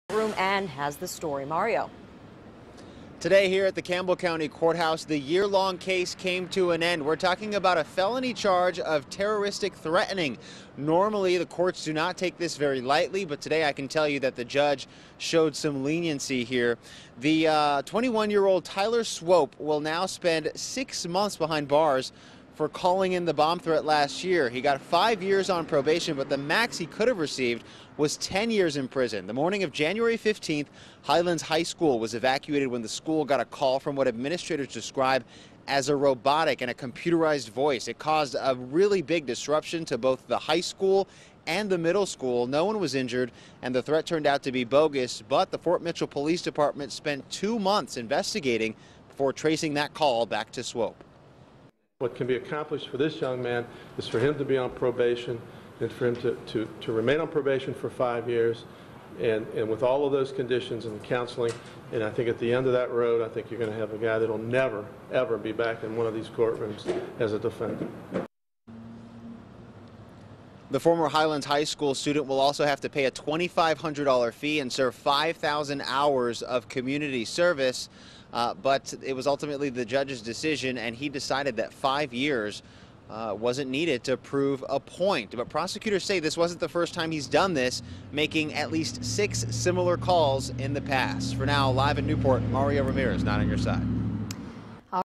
Man sentenced to for phoning in a bomb threat to Highlands High School in January 2014
Tyler Swopes will serve at least six months in jail.